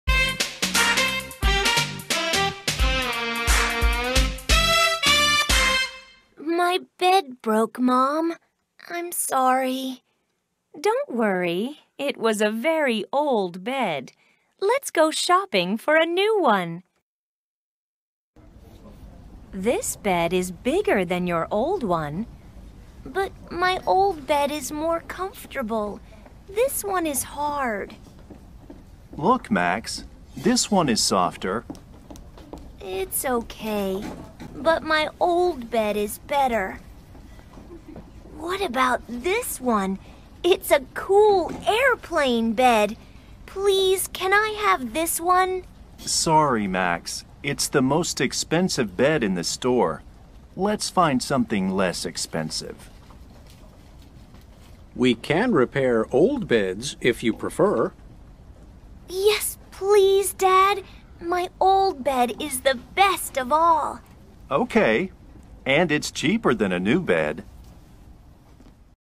Unit 8 The best bed! - Lesson 1 story, SB page 58
Đây là video bài đàm thoại trang 58 , Family and Friends Special Edition Grade 5 – Student book (sách bài học)
👉 Các em xem và luyện đọc to theo ở nhà nhé.